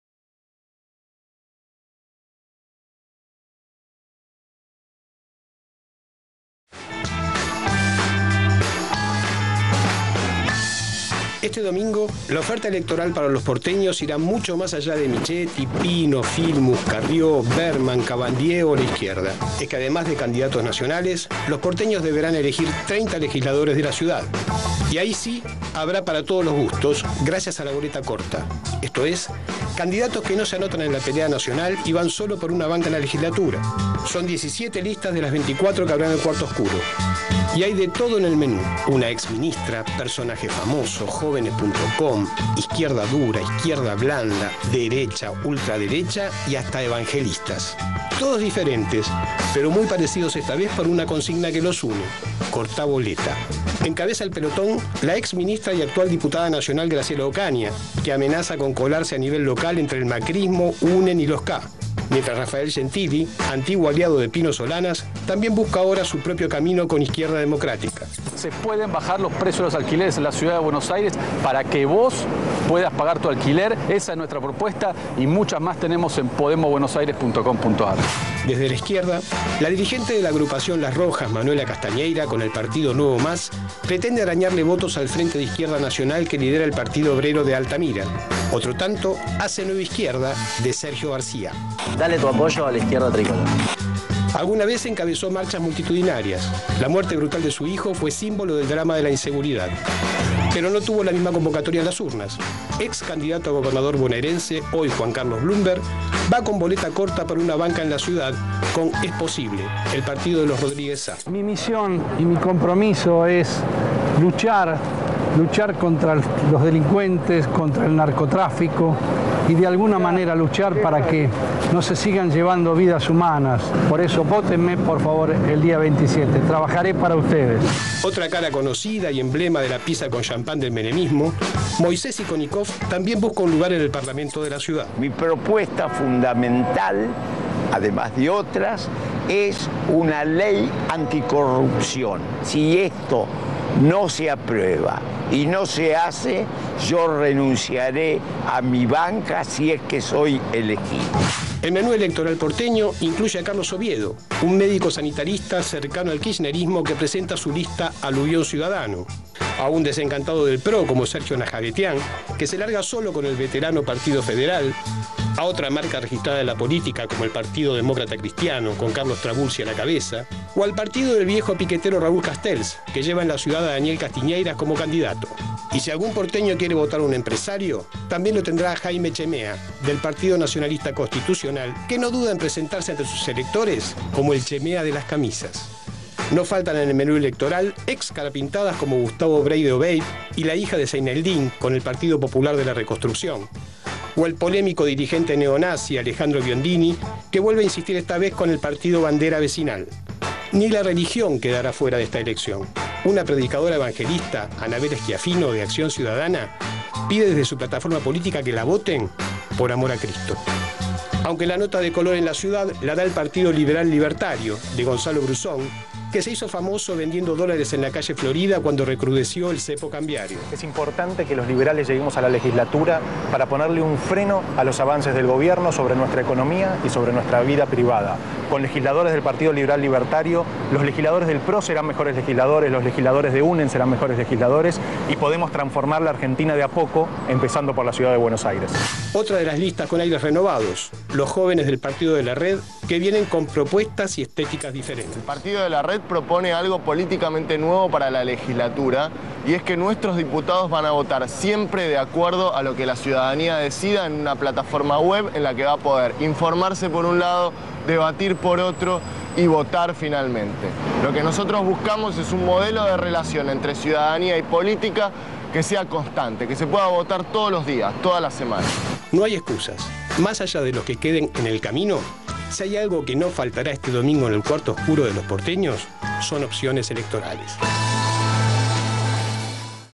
La otra campaña (TN) / 24.10.2013
El 27 de Octubre tenés una alternativa para votar legisladores de la ciudad, cortar boleta por la lista 904.

Soy Rafael Gentili, encabezo un equipo de militantes políticos, sindicales, universitarios y de organizaciones sociales que venimos trabajando hace cuatro años en la Legislatura porteña y que construimos Sumar Izquierda Democrática. Provenimos de diferentes experiencias partidarias (socialistas, intransigentes, peronistas, radicales, entre otras) y tomamos esta decisión porque los problemas de esta ciudad no forman parte de la agenda de las representaciones políticas nacionales.

En los últimos cuatro años, entre otras iniciativas logramos cambiar la traza de la Línea H del subte y modificar el destino de fondos por actividades del subte --como publicidad, alquiler de fibra óptica, etc.--, que formaban parte de negocios privados y ahora se usan para inversión y mantenimiento. También frenamos la privatización del servicio público de bicicletas e impedimos que la Policía Metropolitana utilice pistolas eléctricas (conocidas como Taser).

Sobre la base de esa experiencia, sostenemos que es posible legislar para acceder a una vivienda o tener un alquiler protegido, reconstruir una escuela pública porteña de calidad y con doble jornada, tener transporte público accesible, eficiente, no contaminante y de múltiple oferta, reconvertir los 33 hospitales públicos en centros de atención de última generación, obligar a los gobernantes a gestionar adecuadamente los residuos o lograr el traspaso con los fondos correspondientes de la policía y la justicia para generar nuestra propia política de seguridad ciudadana.

Estudiamos el presupuesto de la ciudad y podemos asegurar que con una legislación adecuada y con legisladores dispuestos a exigir una ejecución transparente es factible lograr estos objetivos. La Ciudad Autónoma de Buenos Aires tiene un presupuesto de 50.000 millones de pesos que se alcanza con lo que pagamos de ingresos brutos, ABL, patentes y demás impuestos, y que año tras año va creciendo. Son recursos que aportamos todos los que vivimos y trabajamos en ella. Queremos que el uso de esos recursos tenga como prioridad mejorar la calidad de vida en la ciudad, ampliar las oportunidades y consolidar los derechos como ciudadanos.

Por estas razones, y porque estoy convencido de que una ciudad más justa, sustentable y amigable es posible, te invito a que cortes boleta y nos acompañes con tu voto.

RAFA GENTILI
Sumar Izquierda Democrática